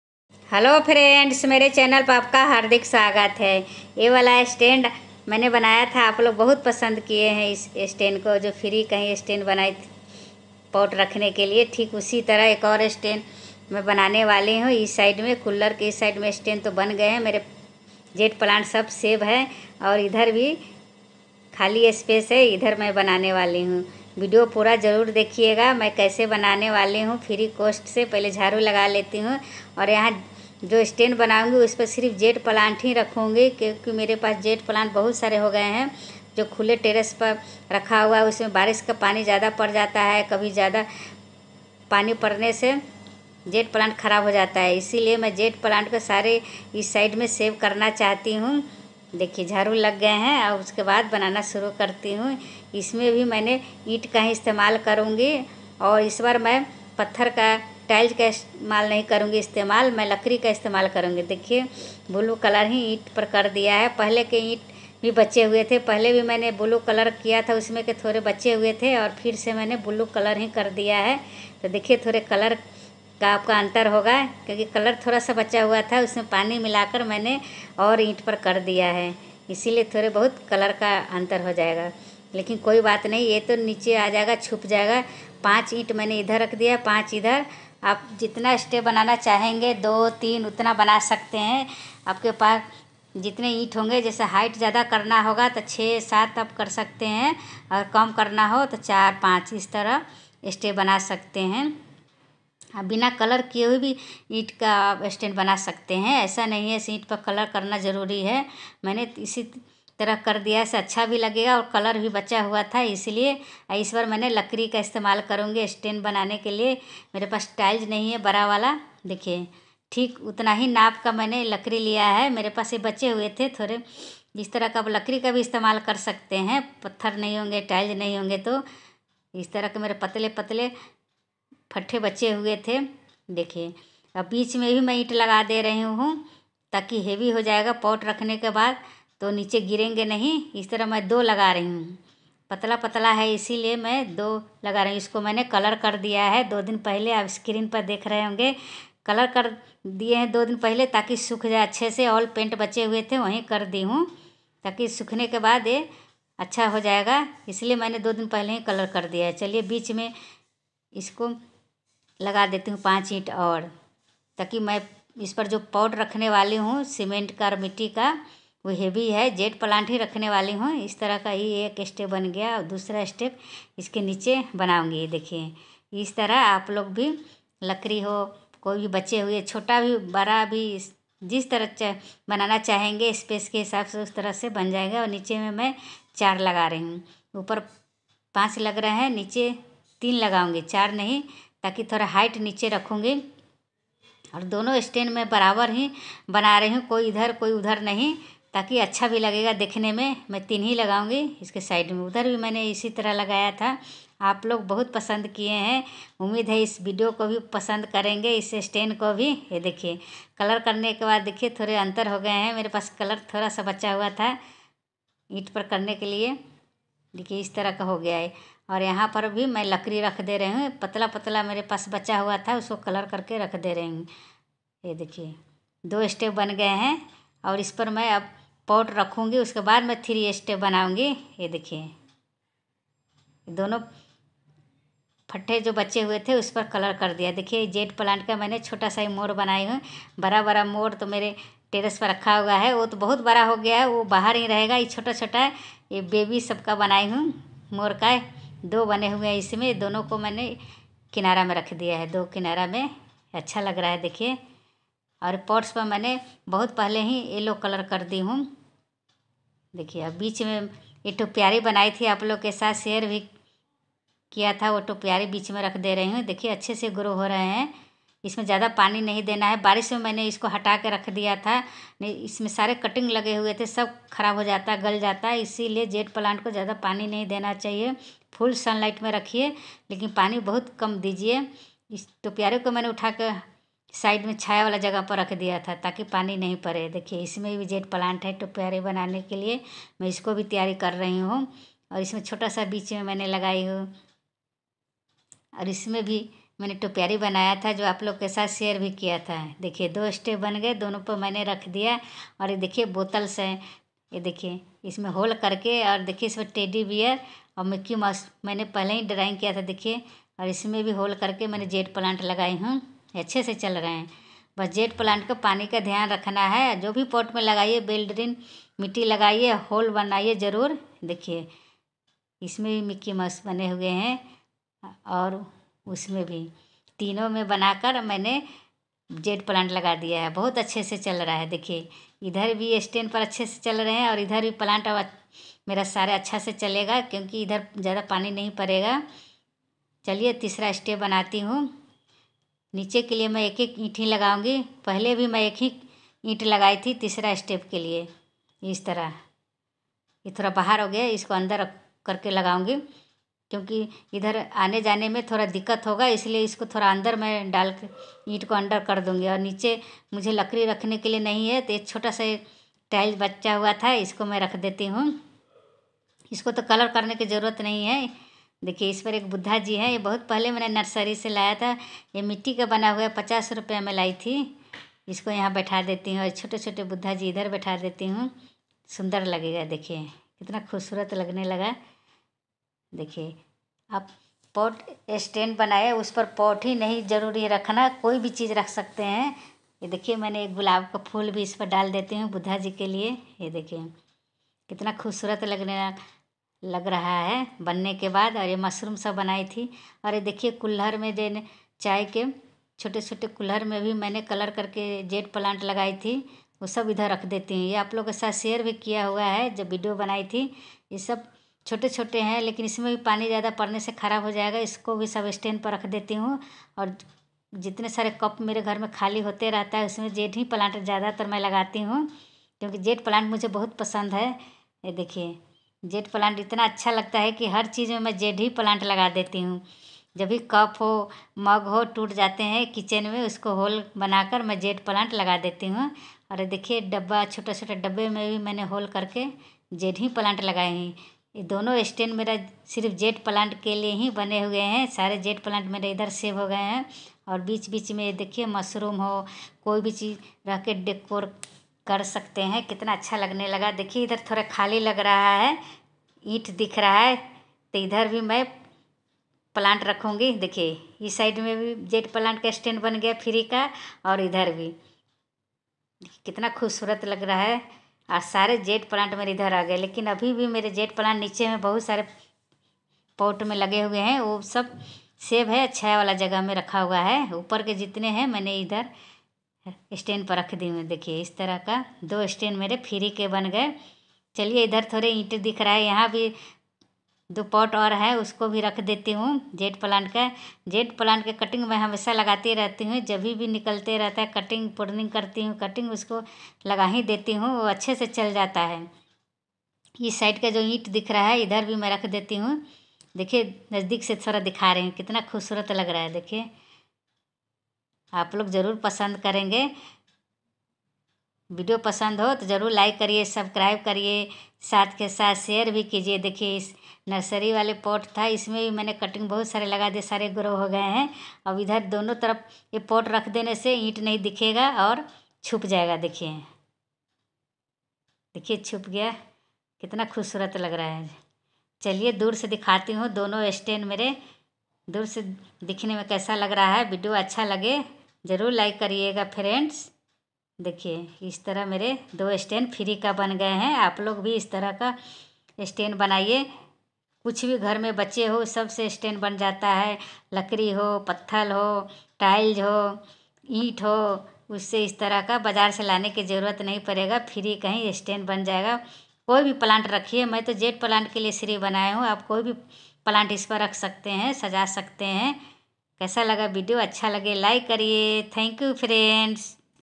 देखिए मैंने फ्री में गमलों का स्टैंड बनाया /पौधों का स्टैंड / Homemade Plant Stand In Free
आज मैंने इस वीडियो में गमलों का फ्री में स्टैंड बनाया है

Plant Stand /पौधों ka स्टैंड
Pot Stand
गमलों का स्टैंड
Homemade Plant Stand In free